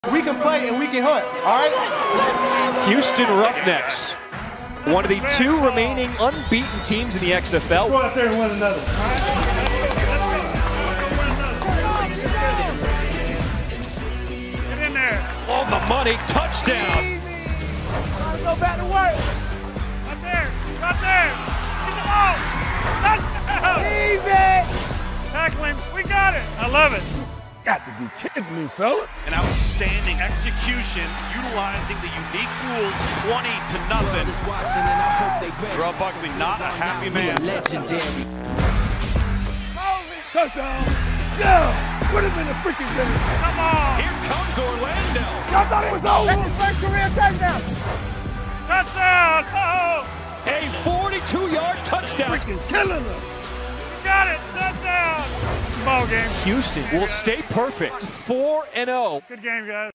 XFL - Mic Drop: Houston Roughnecks vs. Orlando Guardians
Behind-the-scenes of the XFL 2023 Season Week 4 match-up between the Houston Roughnecks and the Orlando Guardians